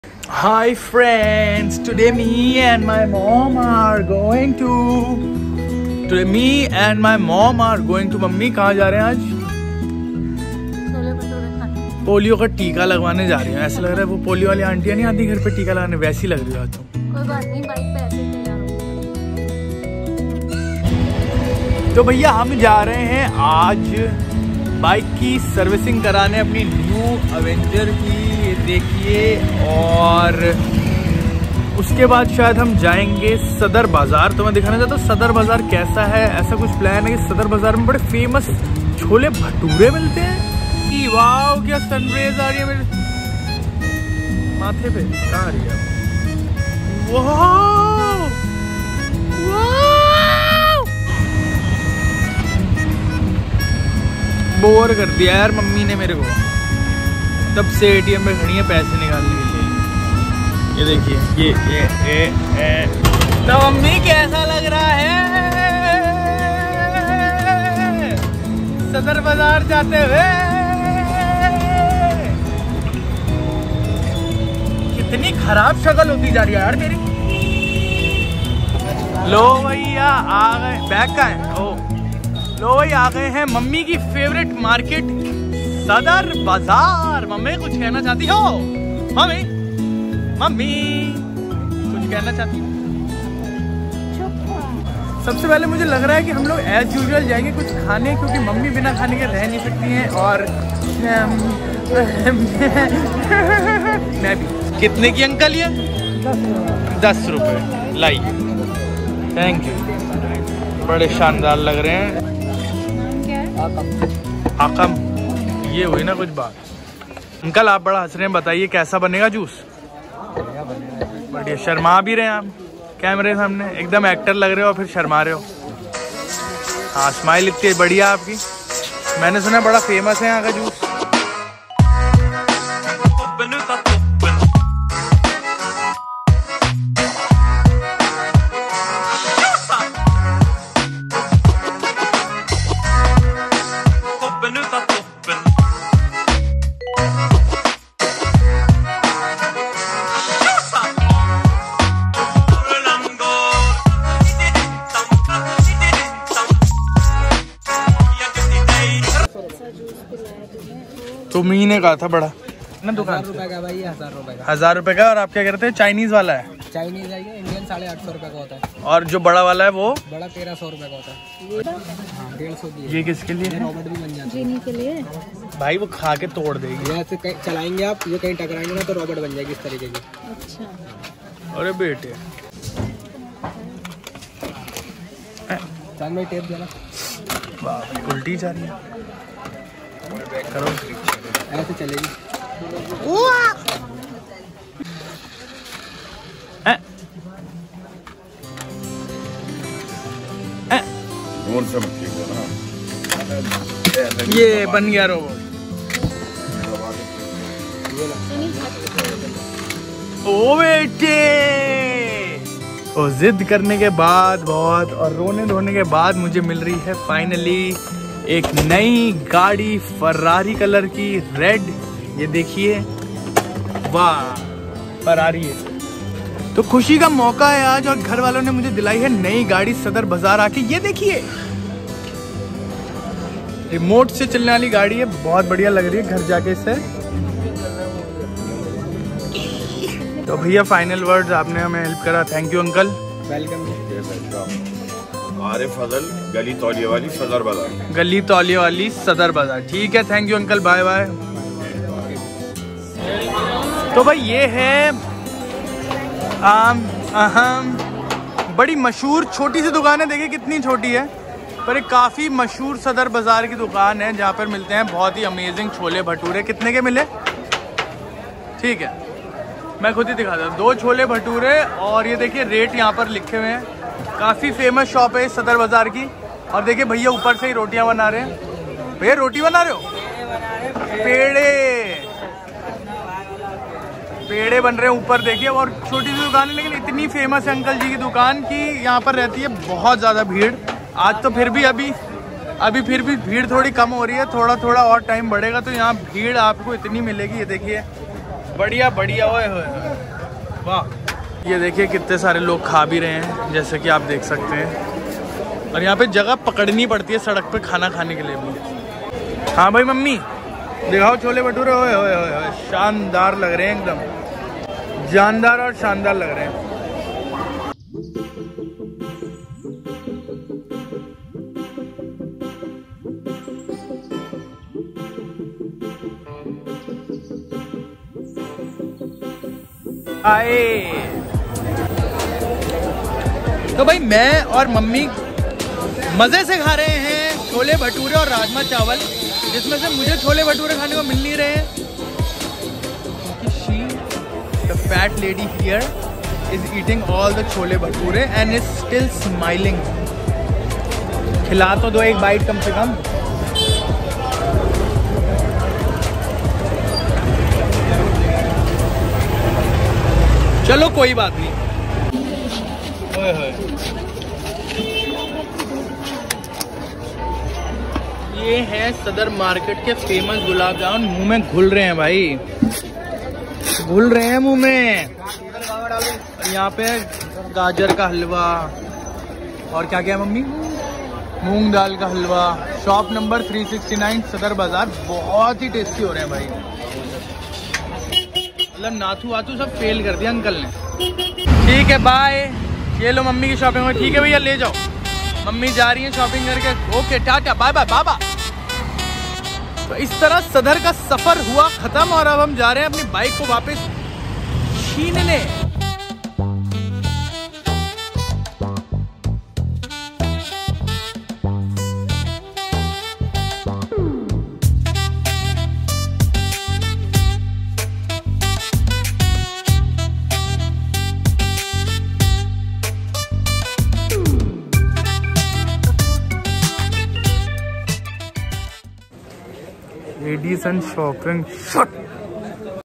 MOM BOUGHT ME MY FIRST CAR😲😍🔥 | Day out with mom ❤️ Sardar bazaar vlog
Finally after years of patience my mom bought me my first car! This is sadar bazaar vlog which takes you through a mom’s love towards her kid , after saving money for almost 29 years my mother out of love bought me red Ferrari coloured car 😍 enjoy the vlog ❤️